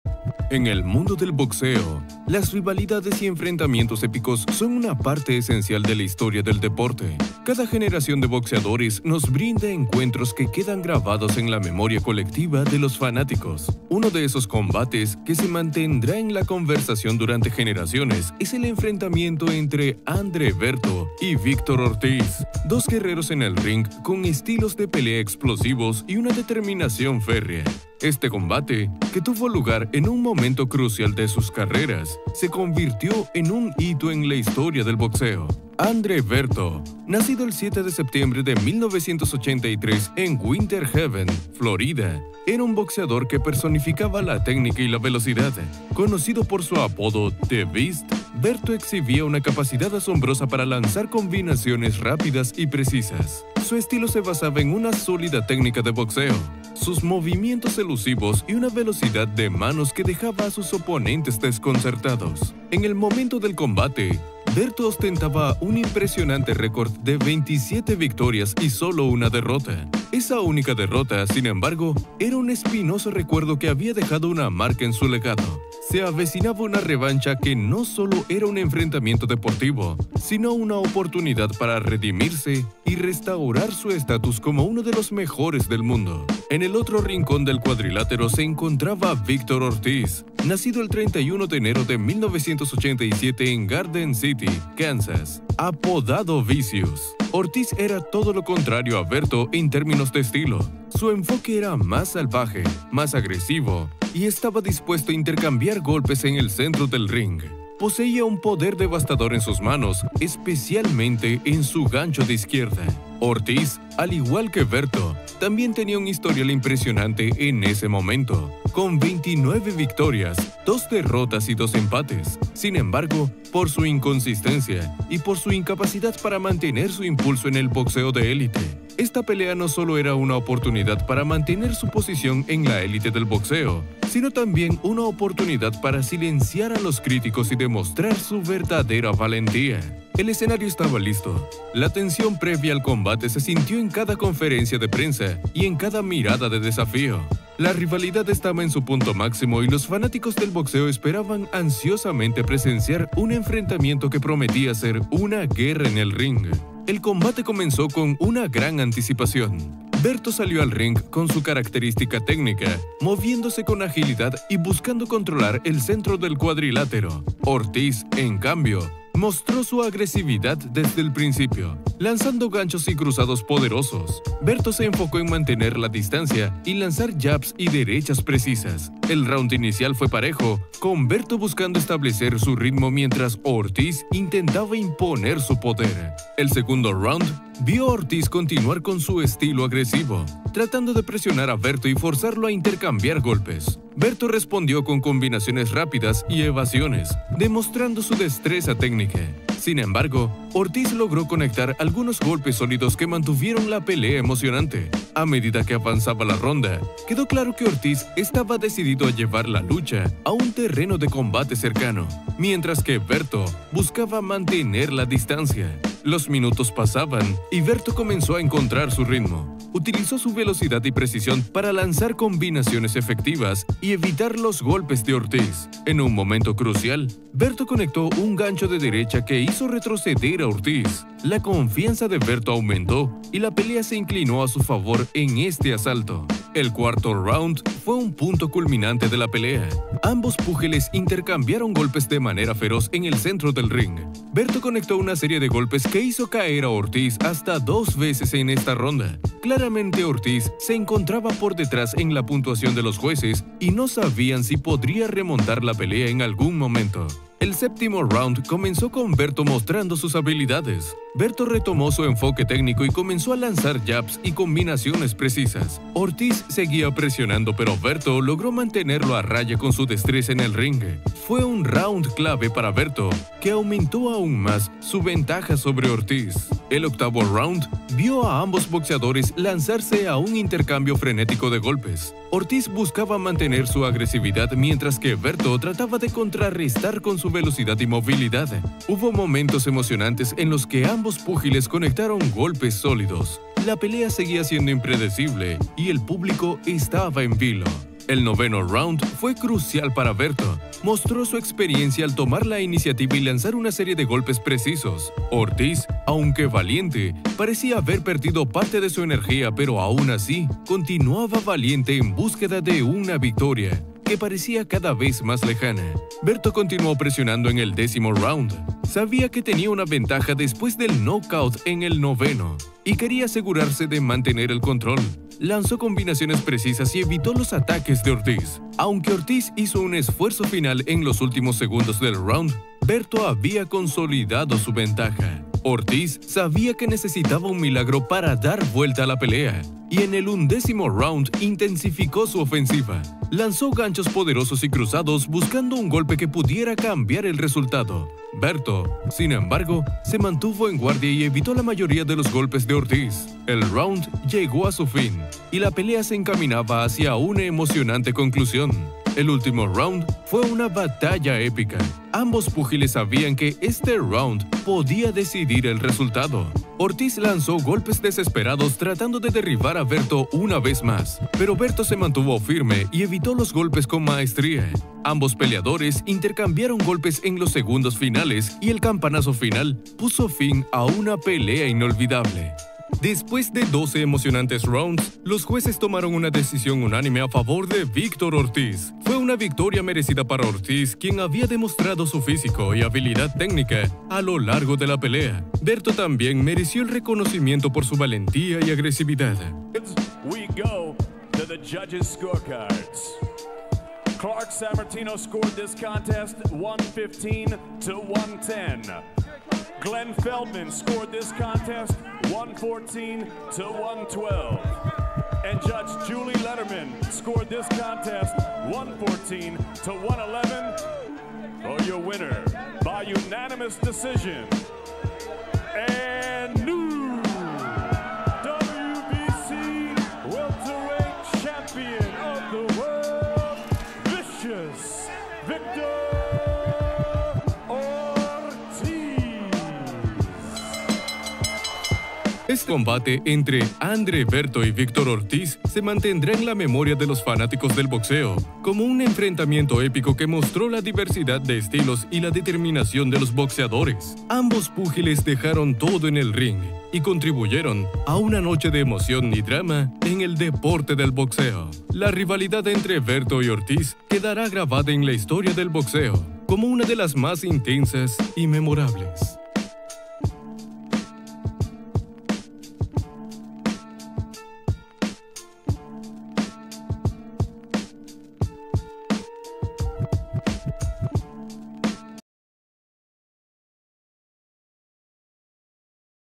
El GRINGO se BURLÓ de MEXICO pero y ACABÓ con su CARRERA | Ortiz vs Berto
Victor Ortiz fue uno de los mayores campeones que el boxeo latinoamericano ha tenido, su combate contra Andre Berto fue una de sus mejores batallas. En el Ortiz vs Berto dos de los mejores boxeadores de la historia se enfrentarían por el título mundial en una batalla que quedó grabada en la mente de todos los espectadores.

Subscríbete para más y deja tu like!!